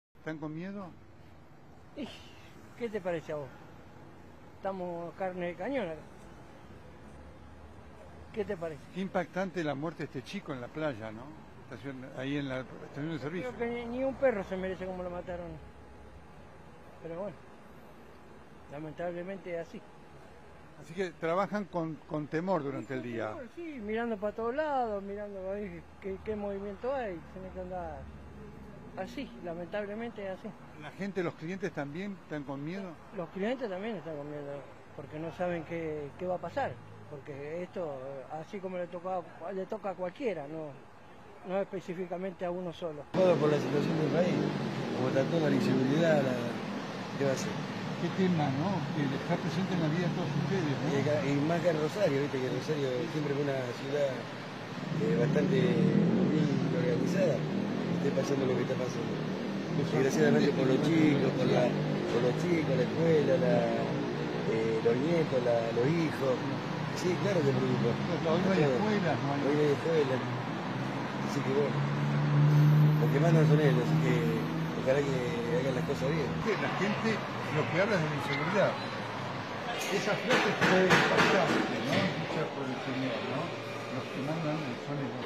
Nelson Castro viajó a Rosario en medio de la dramática escalada del terror narco
El pulso de la vida cotidiana, paralizado por el pánico: "Somos carne de cañón".